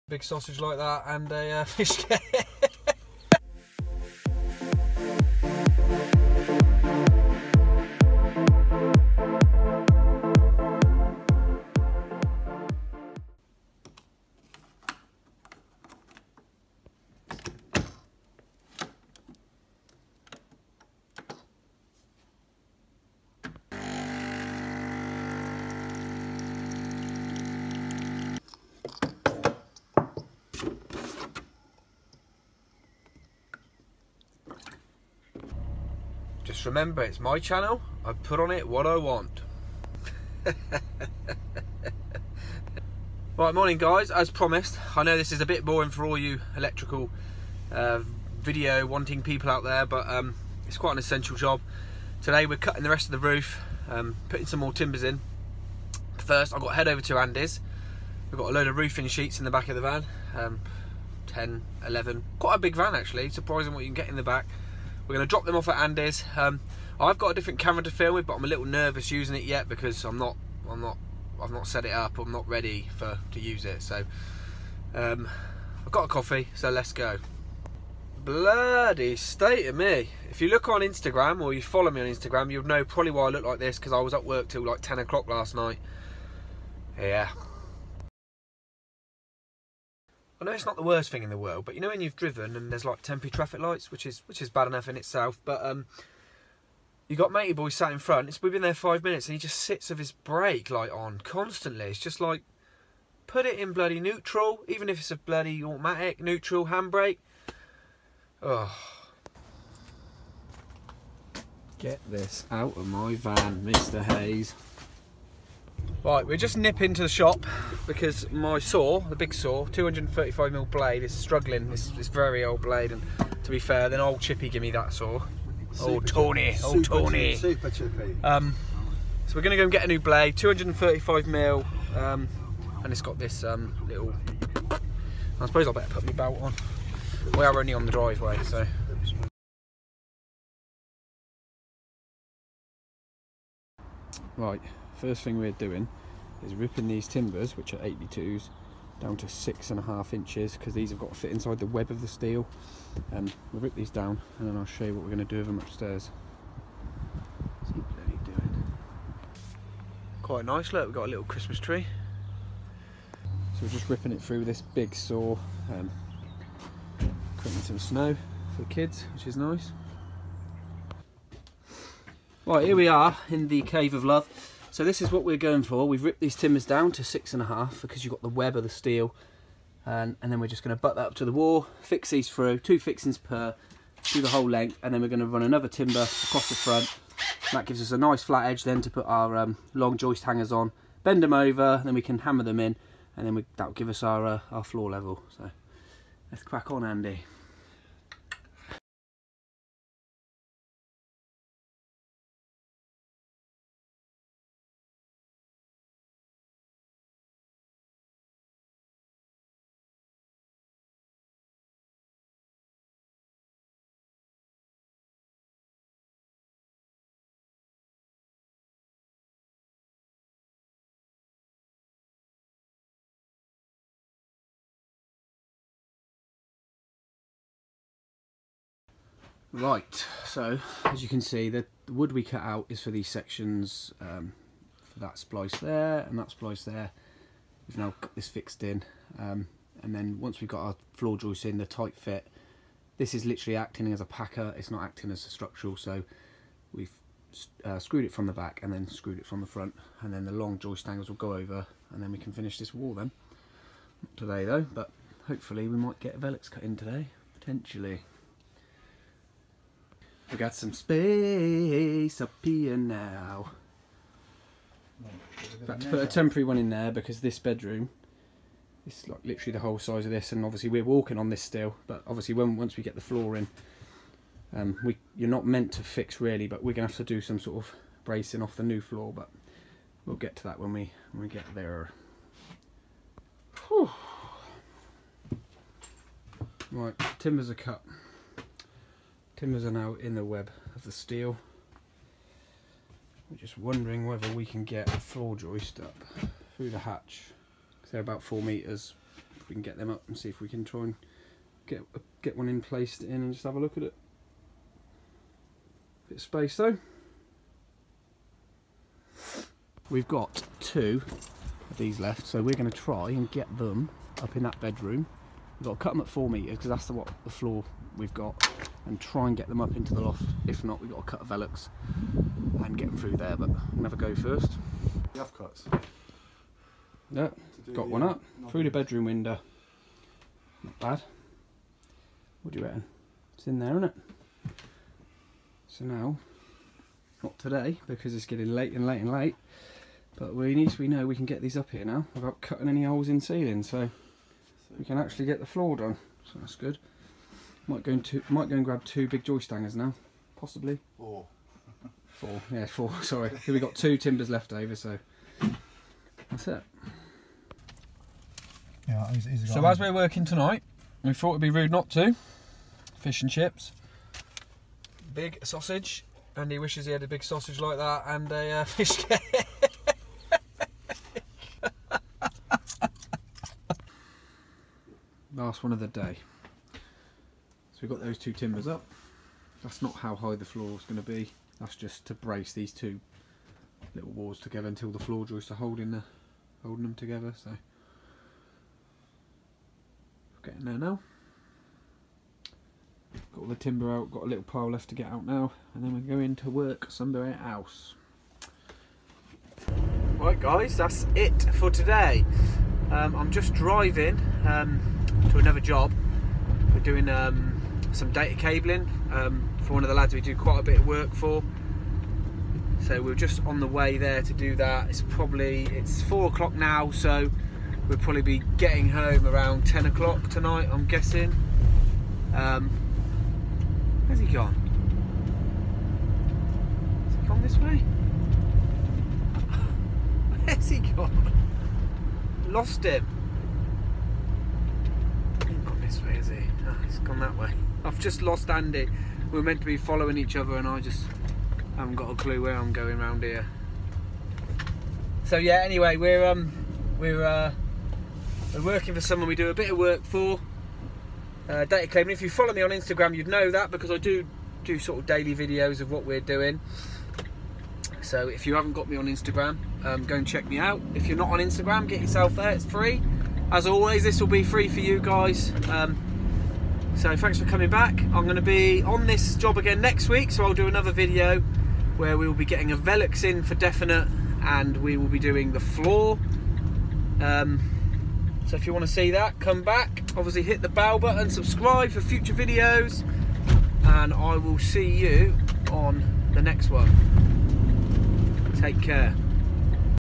Loft Conversion// Existing roof cut and ready for floor.
Loft Conversion part3
Roof cutting and ready to install the new floor joists.